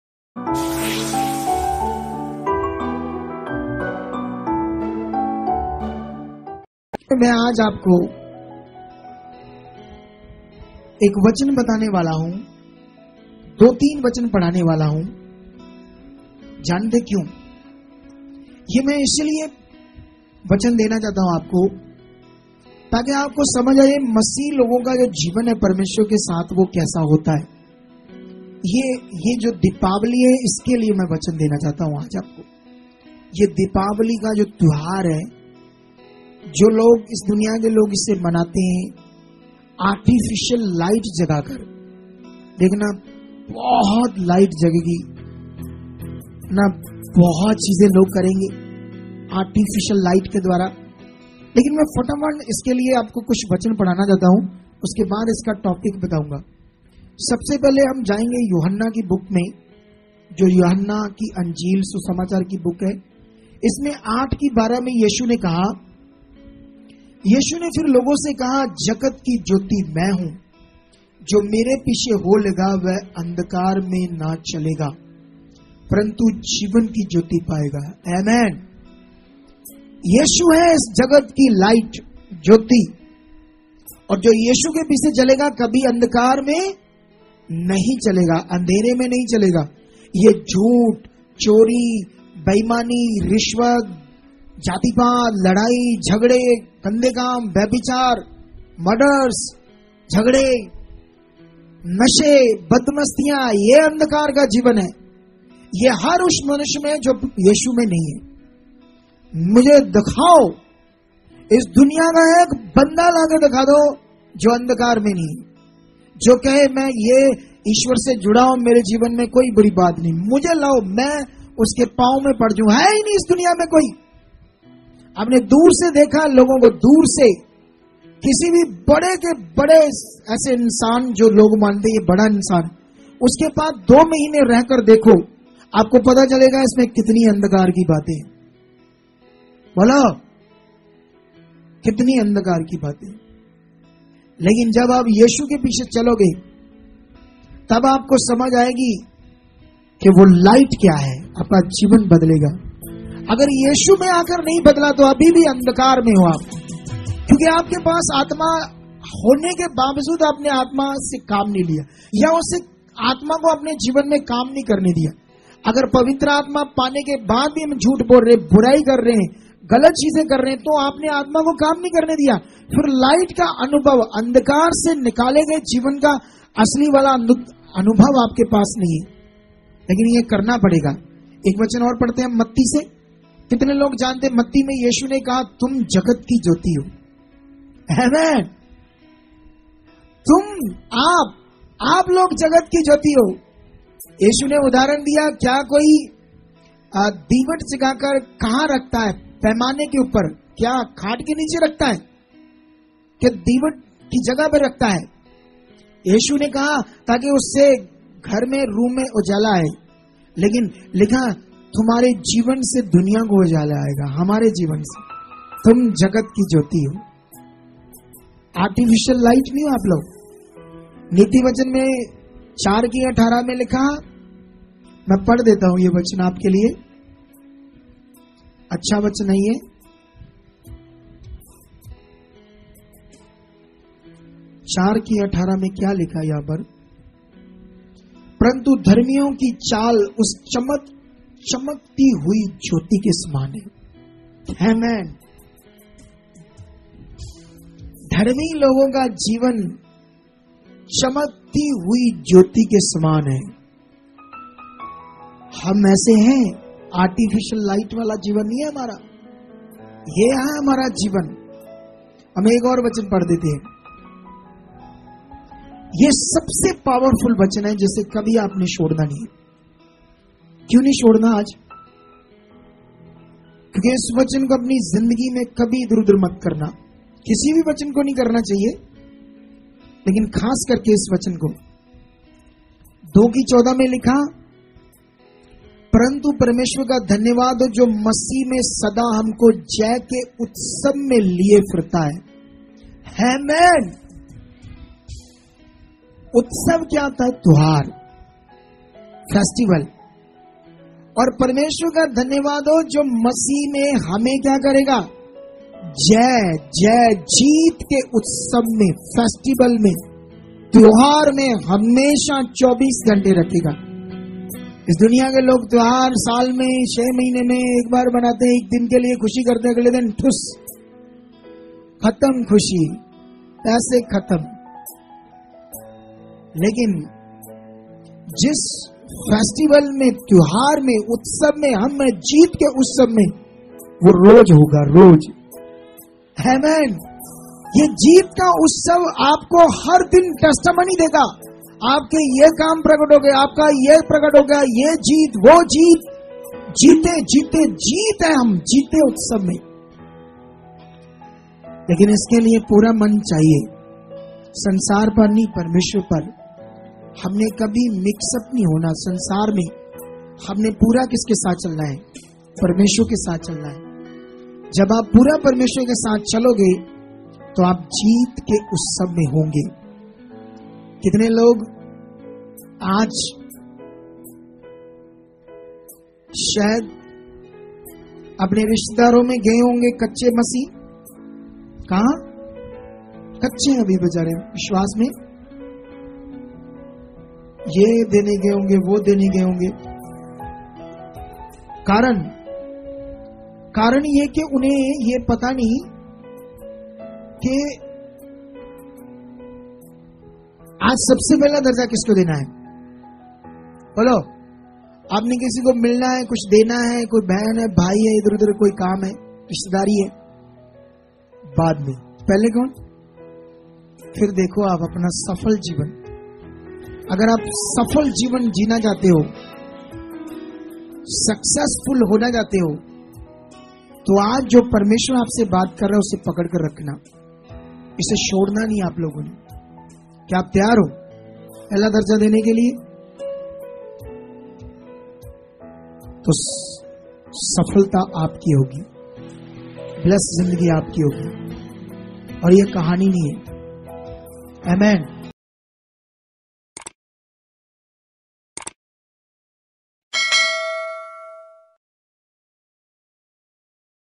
क्या_मसीह_लोग_दीपावली_मनाते_हैं___इसका_क्या_मतलब_है || Do Christians celebrate Diwali?
यूहन्ना 8:12
यीशु ने फिर लोगों से कहा, “जगत की ज्योति मैं हूँ; जो मेरे पीछे हो लेगा वह अन्धकार में न चलेगा, परन्तु जीवन की ज्योति पाएगा।”

       🙏GOD BLESS YOU ALL MASIH FAMILY 🙏
🙏 HALLELUJAH HALLELUJAH HALLELUJAH 🙏